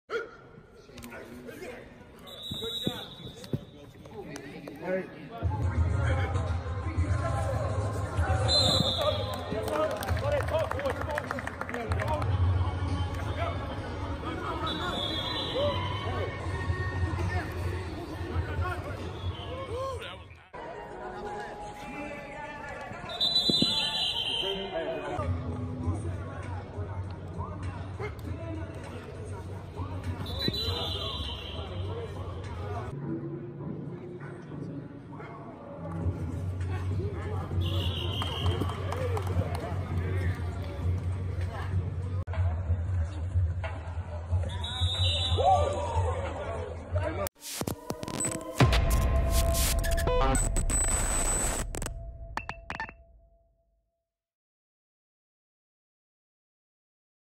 The Opening 2019: Class of 2020 Defensive End Joe Moore Handles Competition
2020 DE: Joe Moore of Cardinal Ritter was our pick for the Nike Opening DL MVP. He dominated with the bull rush, speed and he play DT and won a couple reps as well. Joe’s stock is rising and it should. He looks more locked in than we have ever seen.